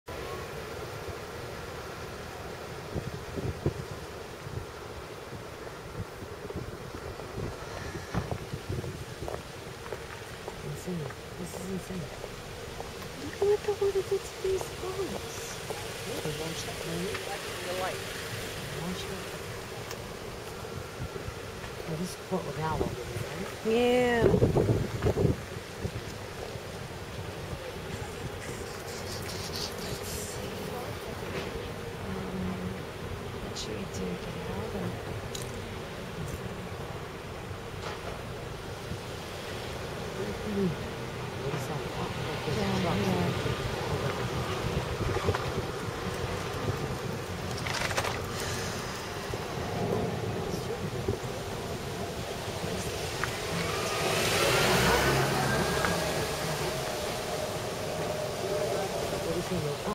Hurricane Sandy aftermath at Great Kills Harbor Staten Island Pt 2
Five days after hurricane Sandy, people in Staten Island came together to help those who had lost everything, those who were cold and hungry.  People started coming from all over with hot food, clothing, blankets, and anything that was needed.  NO Red Cross, no Fema, just us.